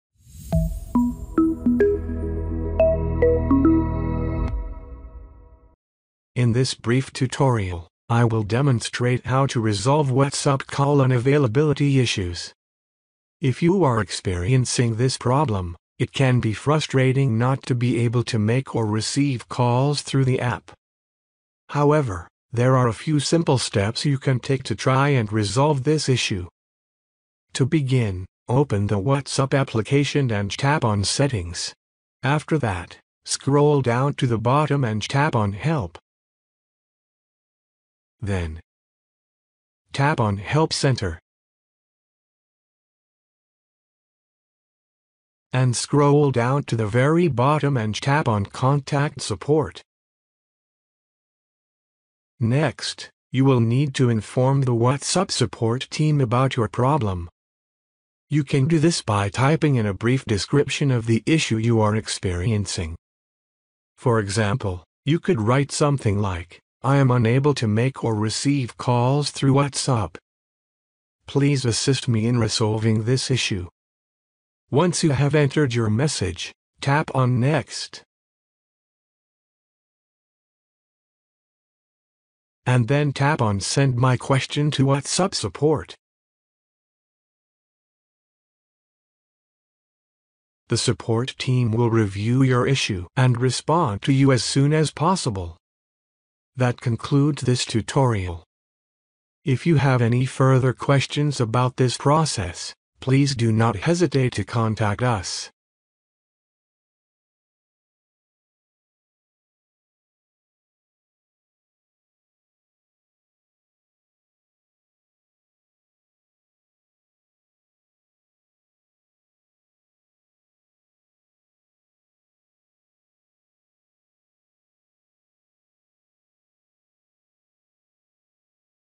How To Fix WhatsApp Call Unavailable
In this tutorial i will show you how you can fix whatsapp call unavalible.